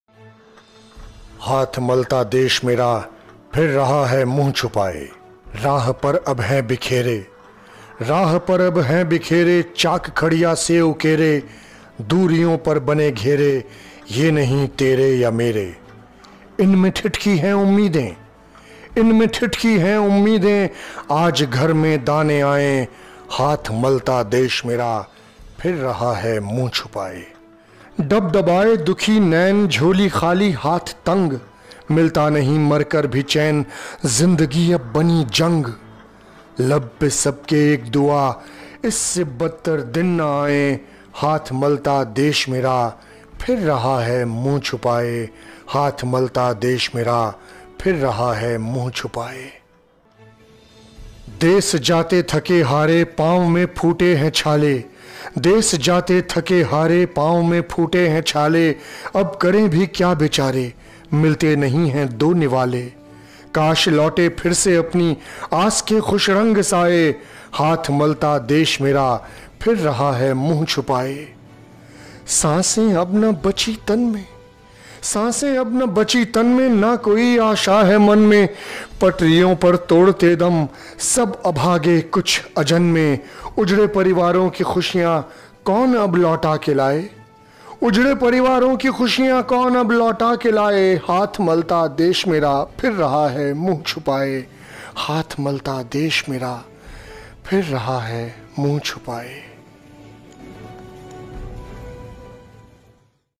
Mask और Sanitizer - Rajat Kulshrestha
मास्क में मुँह छुपाये देश
कैसे कर रहा सैनिटाइज़ है

एक छोटी सी बानगी - रजत कुलश्रेष्ठ की रचना